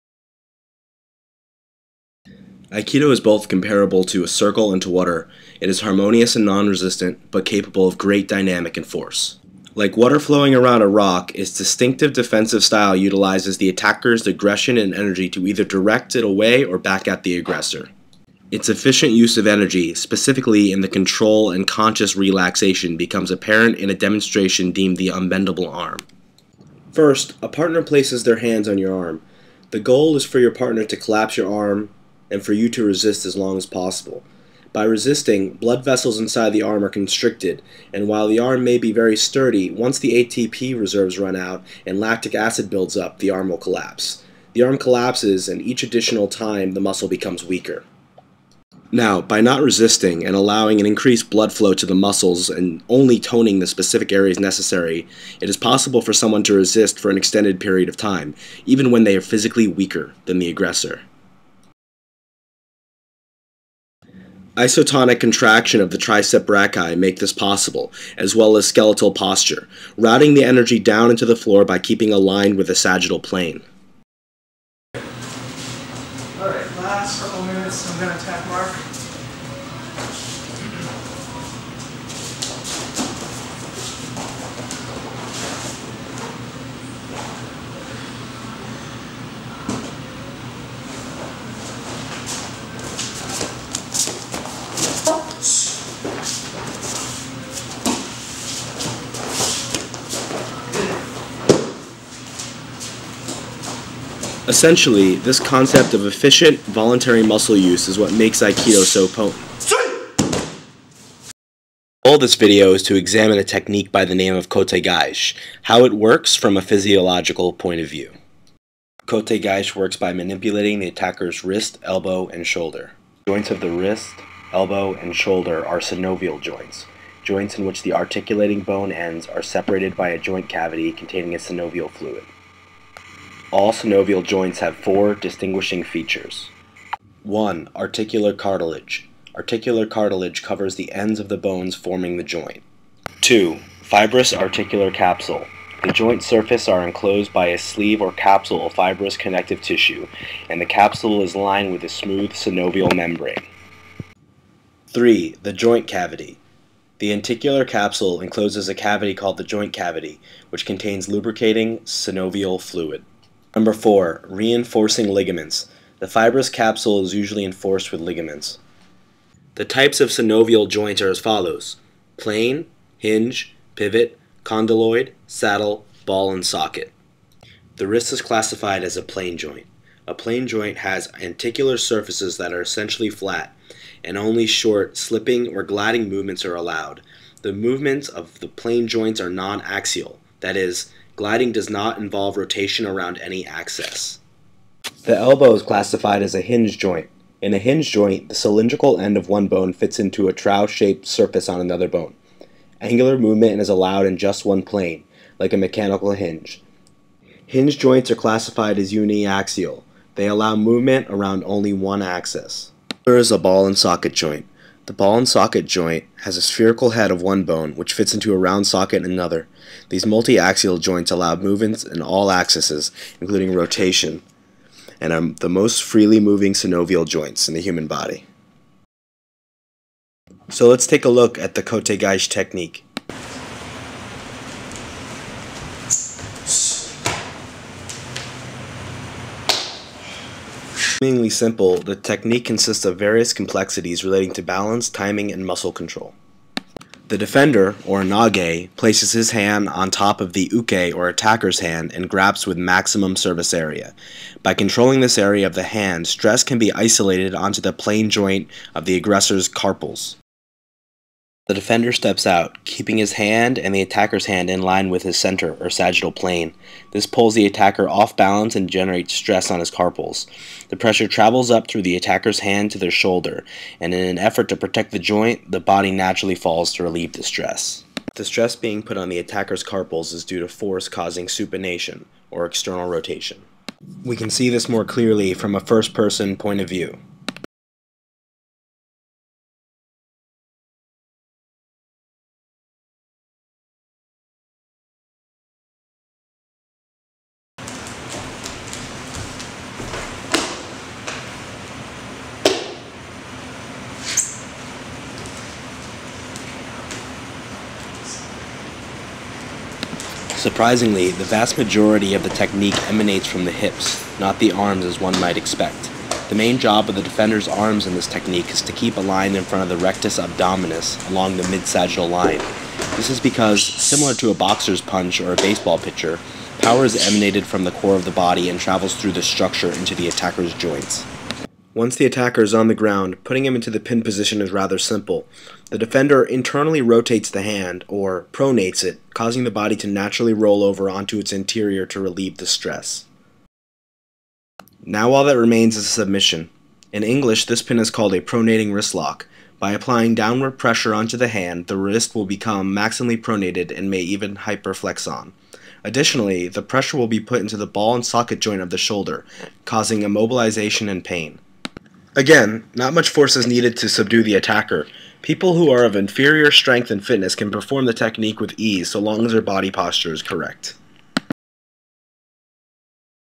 Anatomy of a Circle
Final presentation for Anatomy and Physiology produced by Marc, one of the students at Enso Dojo over-viewing the technique kotegaeshi in anatomical terms.